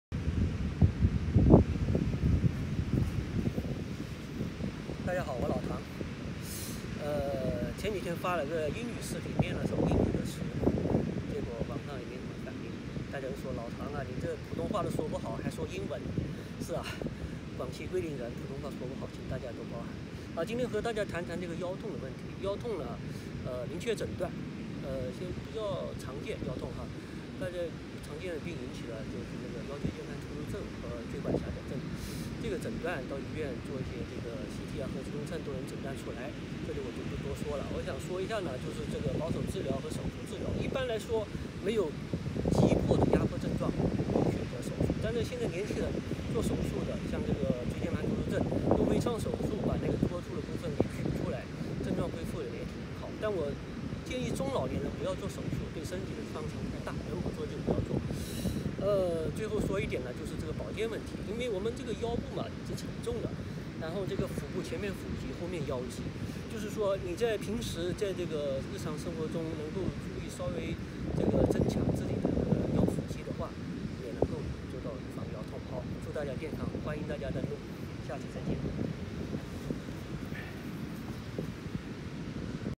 老唐说腰痛那点儿事
腰 腹肌锻练可预防腰痛